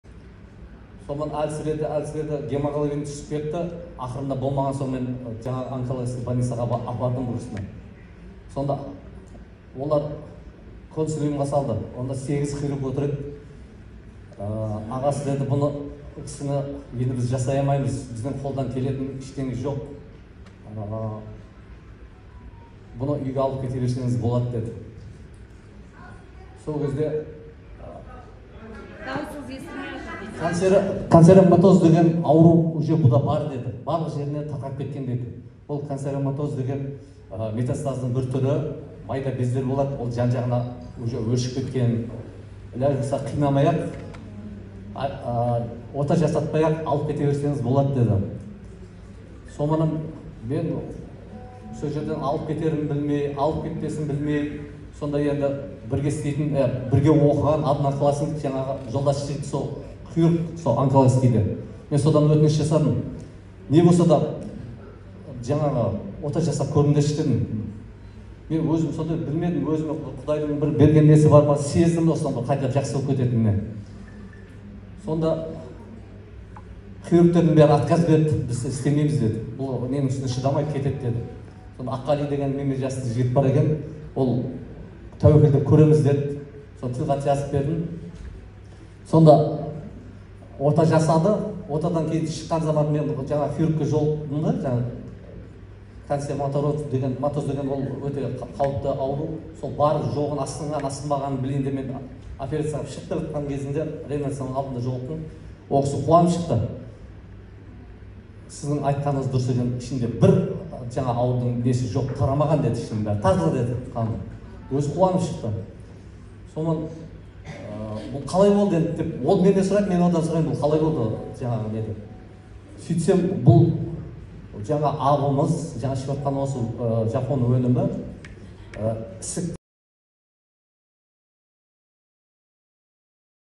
Отзыв о продукции Marine health Онкология
БАДы производятся на основе редких морских водорослей, ламинарий и животных. Сегодня на рынке  достаточно много компании, которые предлагают БАДы, но на рынке все еще нет японской продукции, схожих с нашей. При этом все товары производятся в Японии с применением японской технологии, что говорить о высоком уровне качества товара.   Продукция из Японии обладающий целебными свойствами, прежде всего со стимулирующим и укрепляющим действием. отличные результаты, очень много отзывов, Я всегда искал бизнес, которые приносил бы  людям пользу вид это дает огромную мотивацию. Конечно наша продукция не гарантия от всех болезней, это превентивная мера до того, как они вас настигнут. Я веру, что благодаря БАДам мы сможем изменить продолжительность жизни в наших странах и быть долгожителями как в Японии.
Всем удачи и жду всех в моей команде.